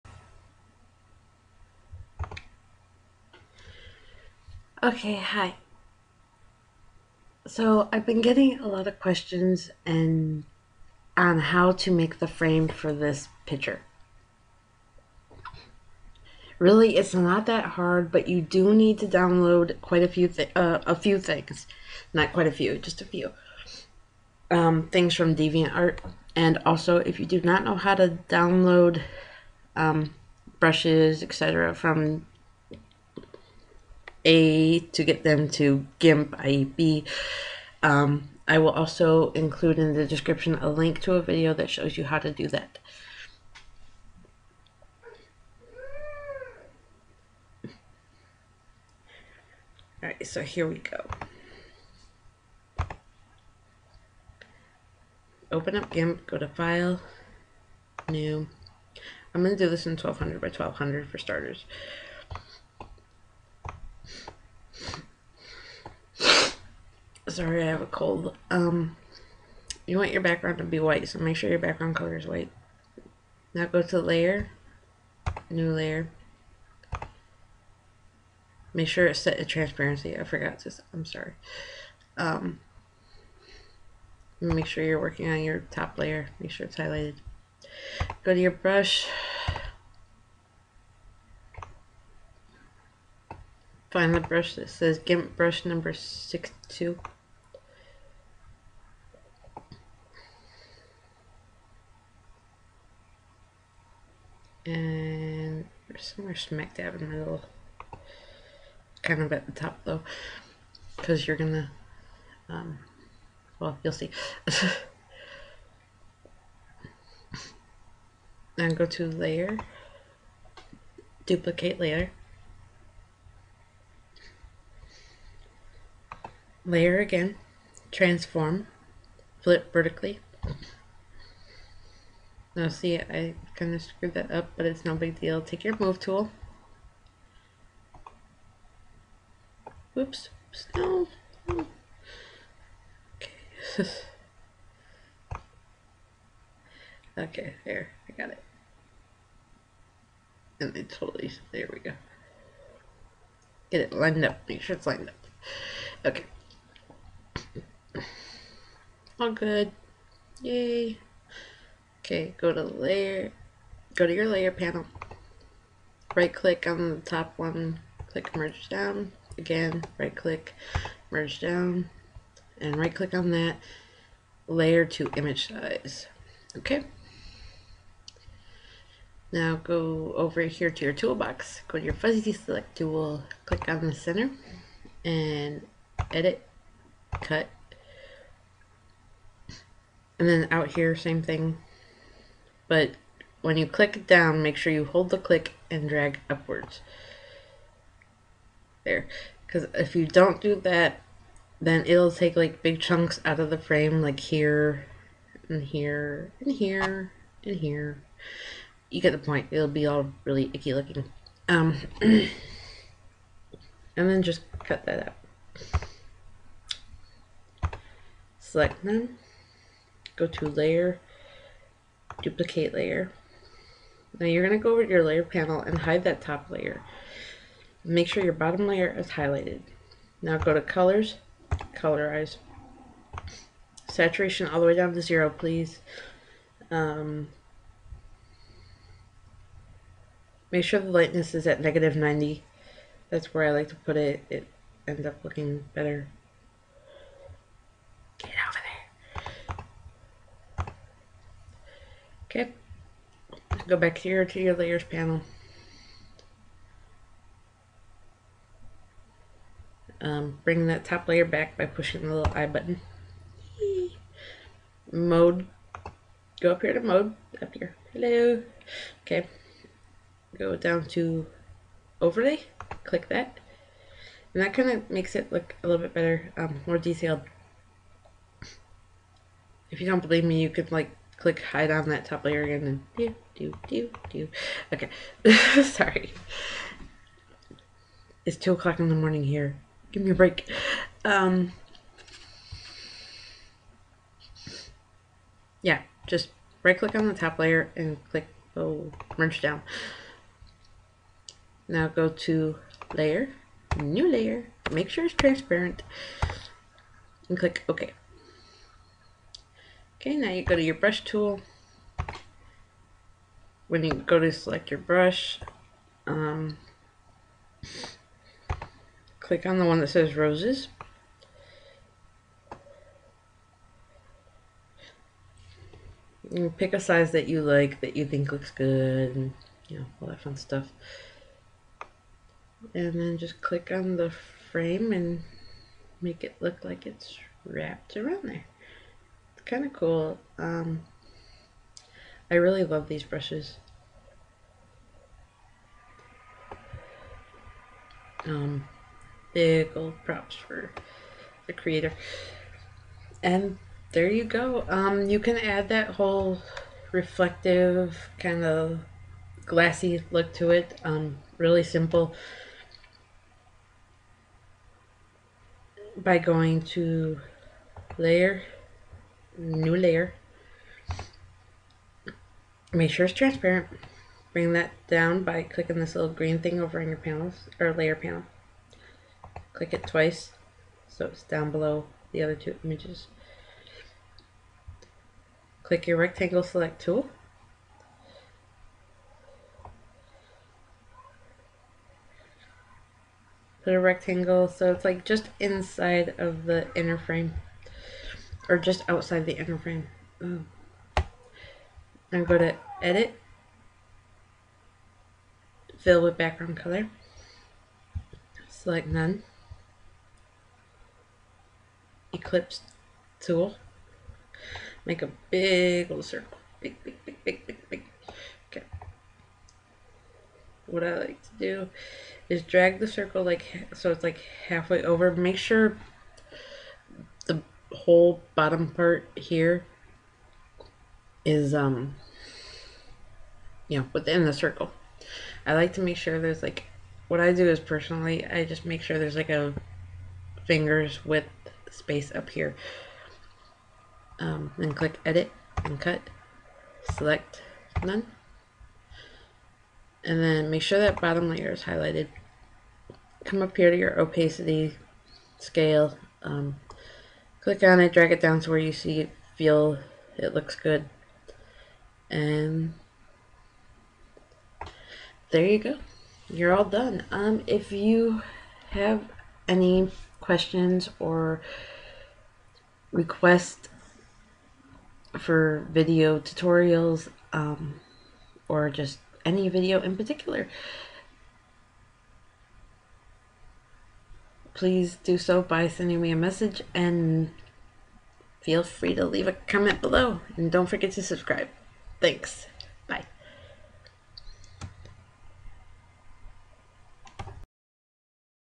Gothic Frame Tutorial ~GIMP
This is a requested Tutorial on how to make a Gothic looking frame using GIMP.

You must download a few items from DeviantArt in order to do this! They are linked below.

Upload these to GIMP (If you do not know how to do this a link to a video on how to do so is provided below).

NOTE: In order for these items to register in GIMP you must restart gimp if it was open during the upload.